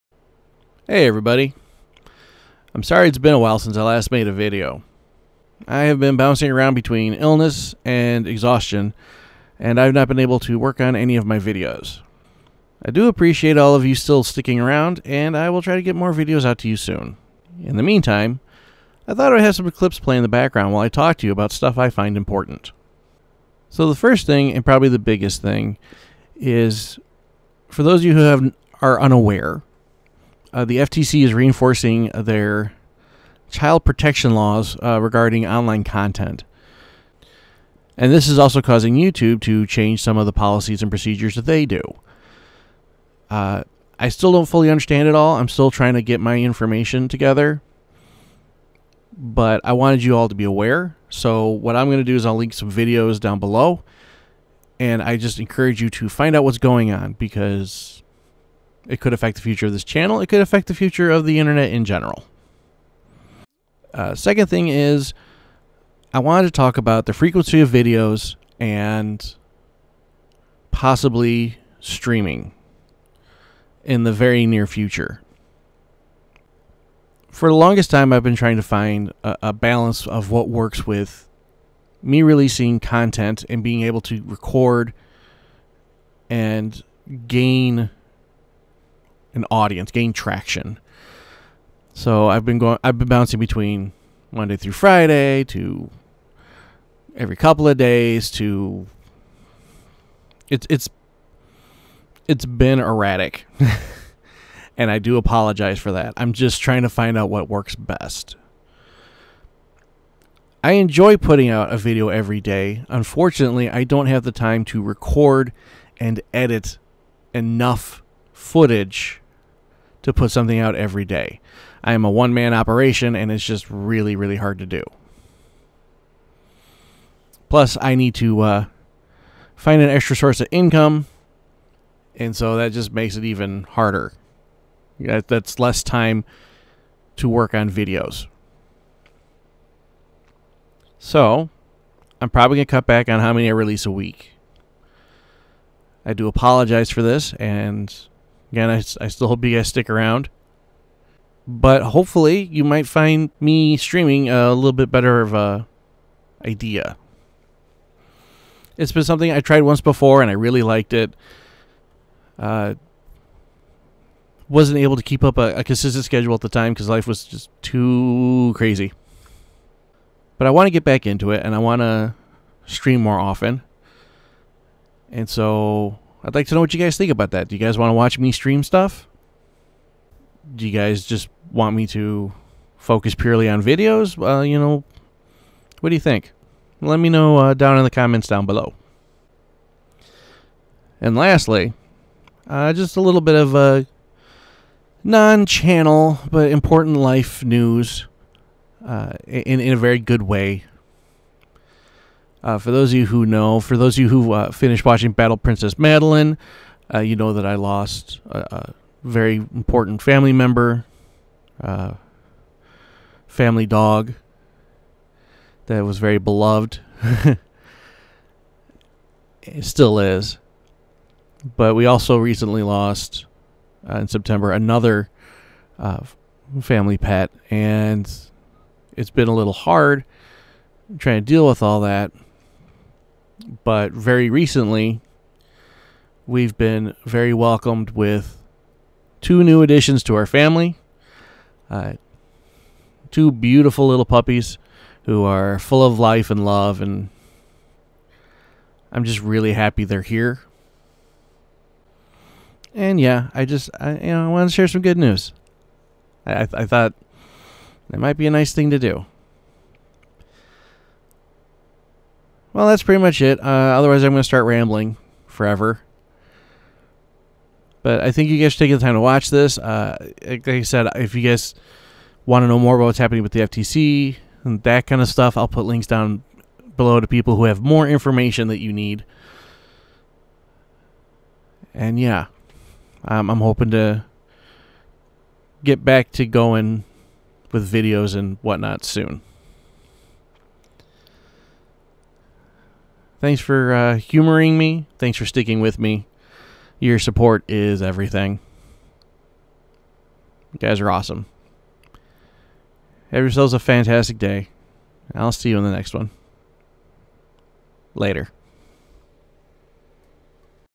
A Quick Update
Sorry I've been missing. Here's a quick update on the present and future of this channel!

There are more links listed on his video, but this is a great jumping off point.

Got something to tell me? Leave it in the comments down below!

You can now check me out on social media!